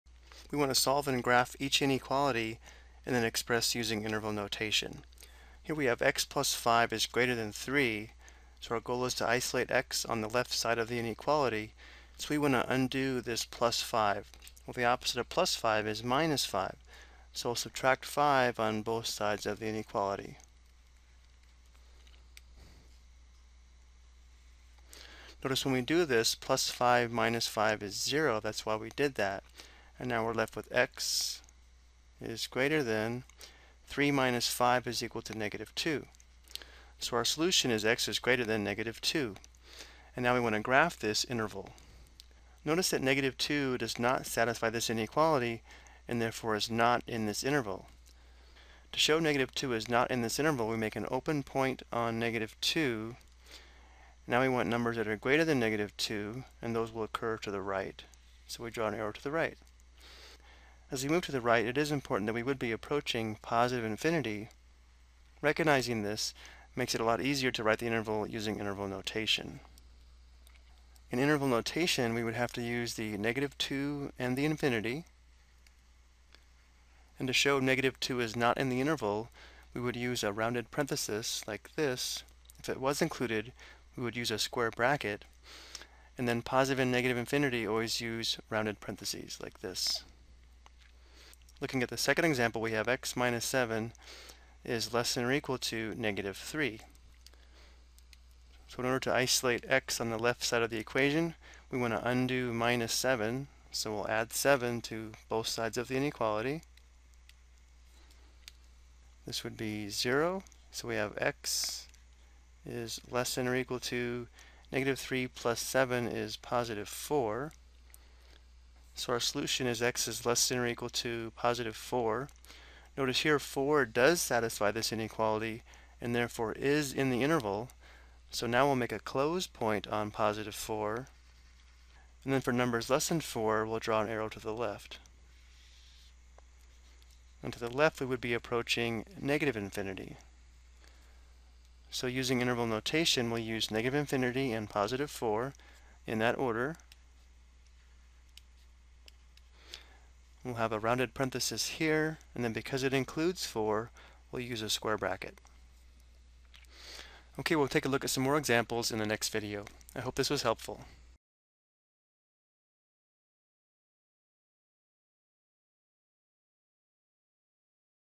Ex: Solving One Step Inequalities by Adding and Subtracting (Variable Left Side)
This video explains how to solve one step linear inequalities in one variable with the variable on the left side.  The solution is graphed and expressed using interval notation.